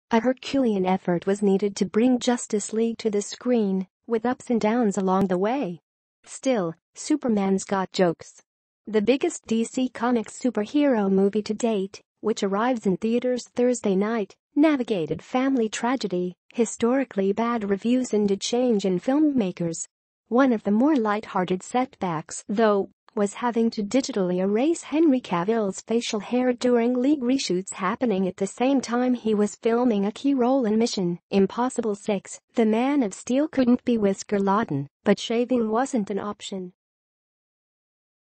How 'Justice League' weathered supersized setbacks to assemble DC's greatest heroes
A Herculean effort was needed to bring Justice League to the screen, with ups and downs along the way.

Still, Superman’s got jokes.

The biggest DC Comics superhero movie to date, which arrives in theaters Thursday night, navigated family tragedy, historically bad reviews and a change in filmmakers. One of the more lighthearted setbacks, though, was having to digitally erase Henry Cavill’s facial hair during League reshoots happening at the same time he was filming a key role in Mission: Impossible 6: The Man of Steel couldn’t be whisker-laden, but shaving wasn't an option